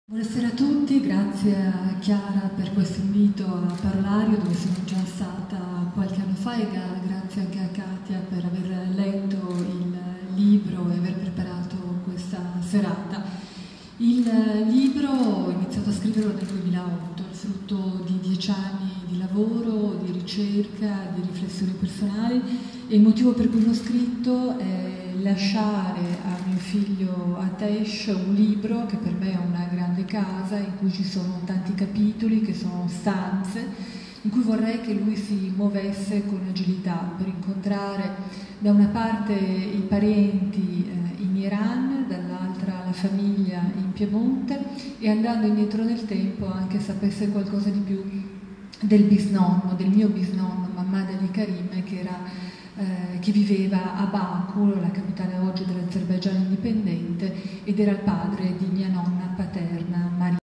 Farian Sabahi a Parolario 2018
La giornalista Rai, di padre iraniano, racconta nel suo primo romanzo autobiografico, l'originalità di un'infanzia vissuta in seno alla media borghesia piemontese, essendo figlia della prima coppia multietnica di quella comunità, negli anni '70.